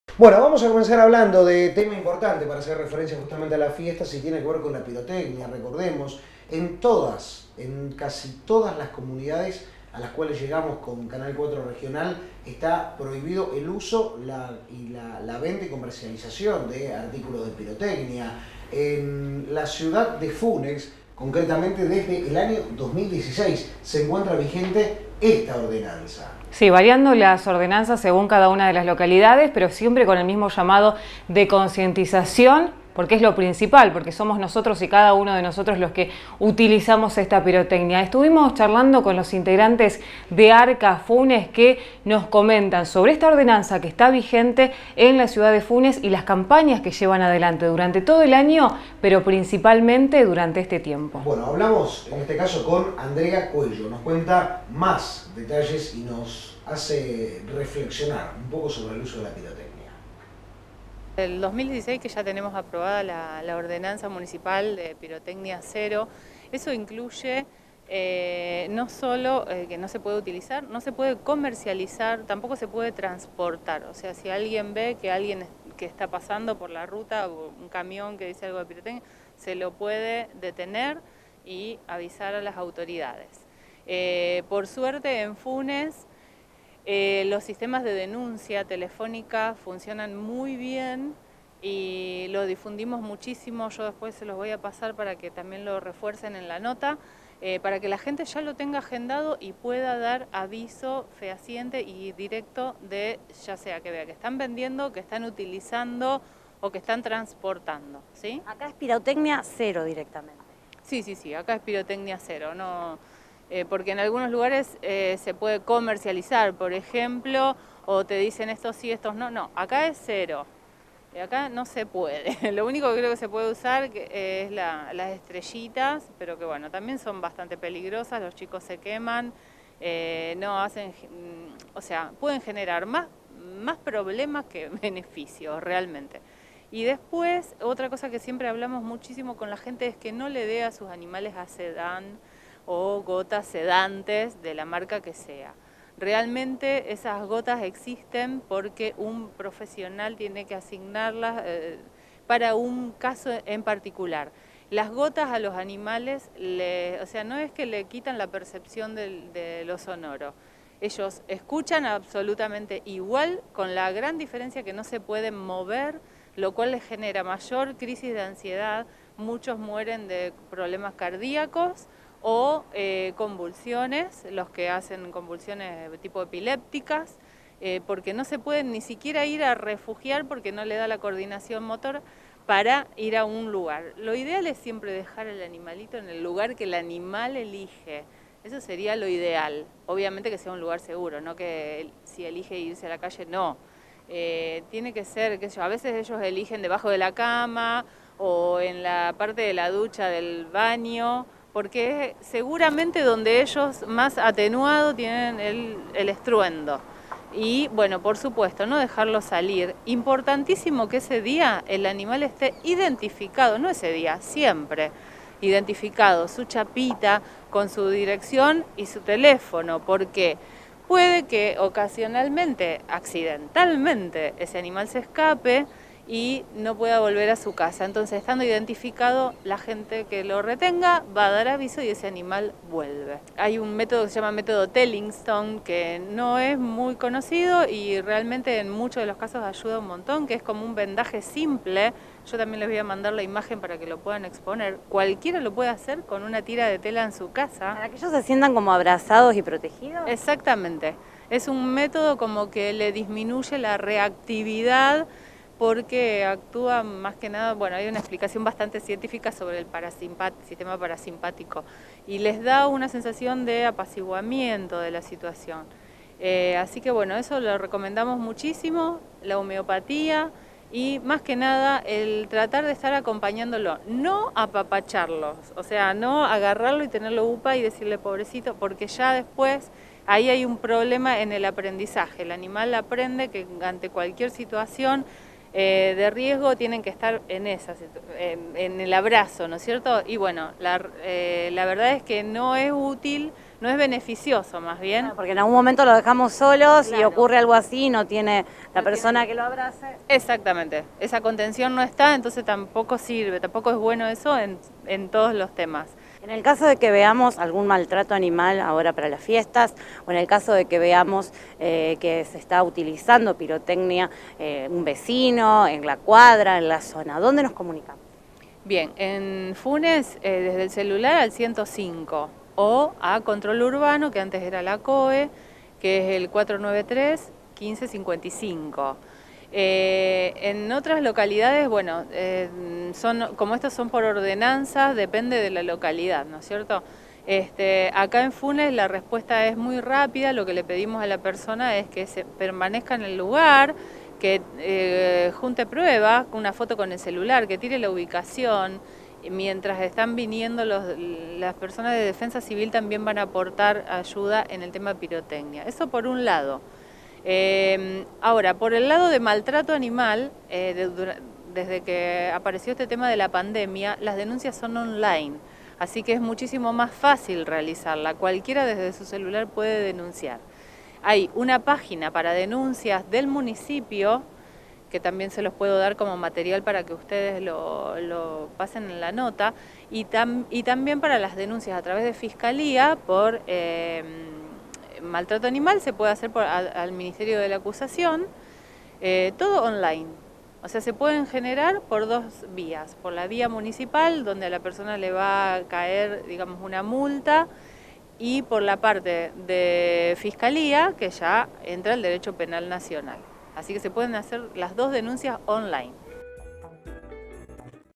Campaña de Pirotecnia Cero en todo Funes
La ONG Arca Funes es una de las instituciones que lleva adelante la campaña de Pirotecnia Cero en las inminentes festividades de fin de año.